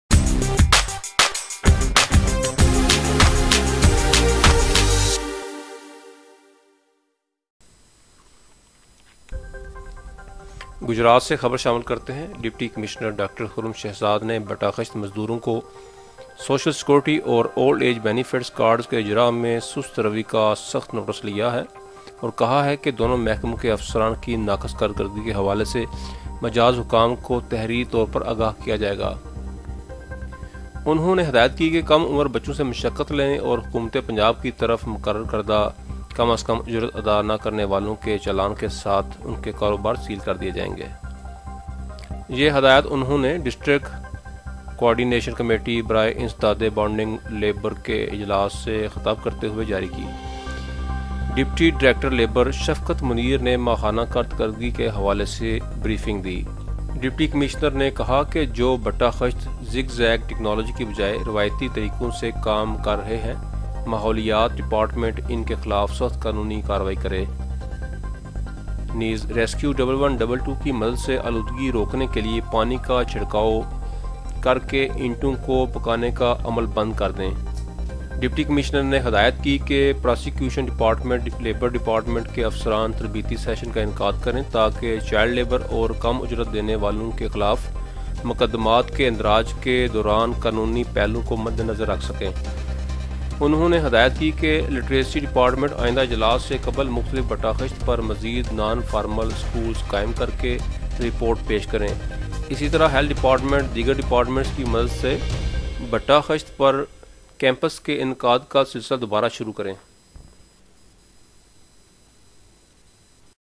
bhutta khisht mazdooron ko social security aur old age benefits cards ke ijra hamza mein sust rawi
بھٹہ خشت مزدوروں کو سوشل سکیورٹی اور اولڈ ایج بنیفیٹس کارڈز کے اجرا ء میں سست روی کا سخت نوٹس
bhutta khisht mazdooron ko social security aur old age benefits  cards ke ijra hamza mein sust rawi ka sakht notice
Strict notice on slow issuance of Social Security and Old Age Benefit Cards to kiln workers
Insaf News Agency انصاف نیوز ایجنسی